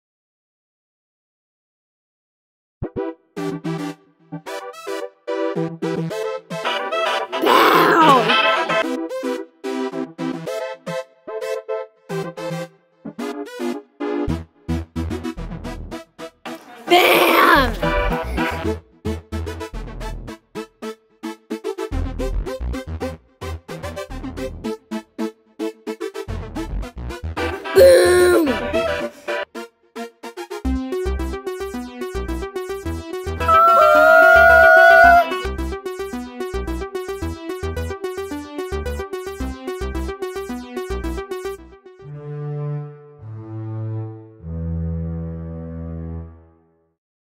Background Music Project: New Super Mario Bros
Fifth grade students explored the effect of background music in movies, video games and commercials.  They then chose clips that represented their interests and create background music and sound effects to represent the clip.  Students used Garage Band on iPad minis to create the music, then merged the music with the video on iMovie.  For more information about this project, please see my